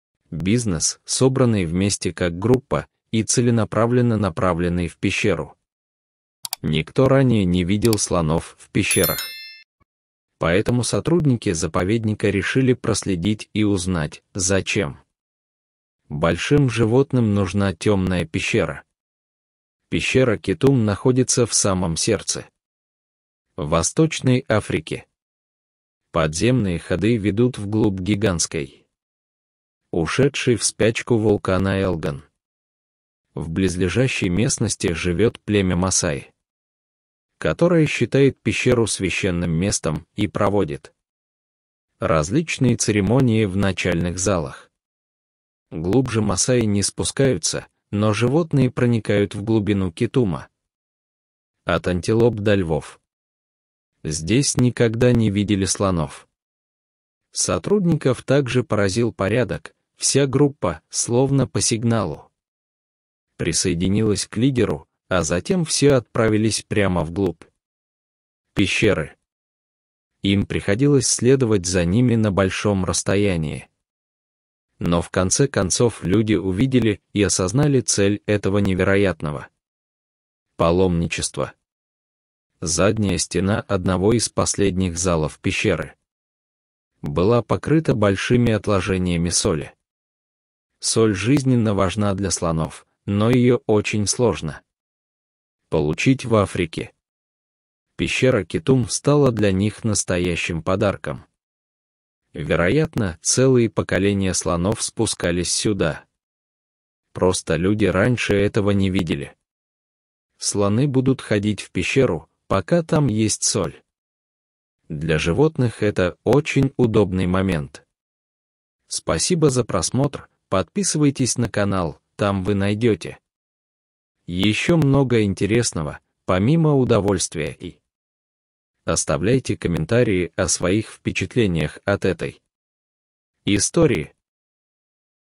Каждую ночь слоны отправляются в пещеру! Люди решили их проследовать! Не могу поверить!
В окрестностях горы Эльгон стражи природы наблюдали большое скопление африканских слонов. В какой-то момент люди заметили, как слоны прервали свои занятия, собрались в группу и направились целенаправленно в пещеру. Никто никогда раньше не видел слонов в пещерах, поэтому стражи природы решили отследить и выяснить, что великим животным нужно в темном подземелье.